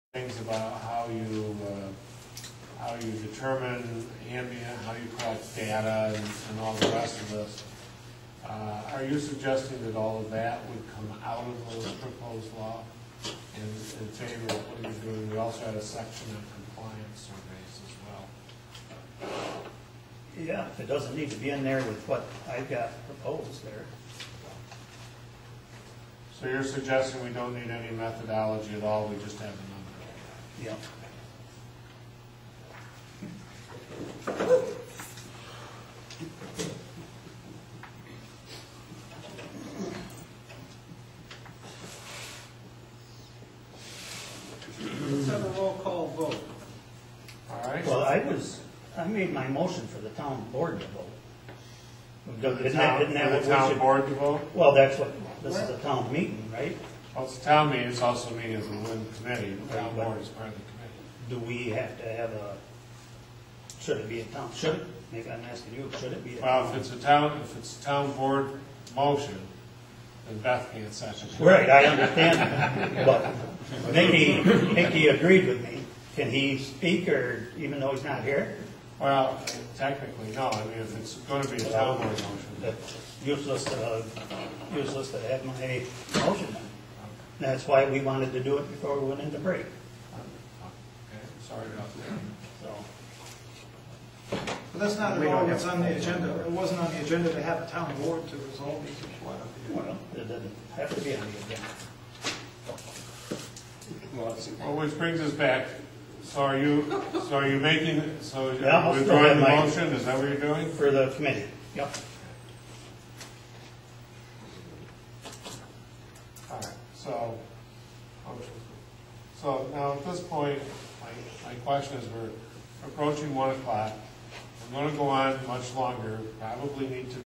Donald Mason attempts to gut the wind law
Donald Mason wants to throw out years of studies, tens of thousands of tax dollars to get a gutted wind law.  He states publicly that he wants to "Help the land owners out with the money they will get." and he does not want to loose a lot of turbines, and his reasons are monatary.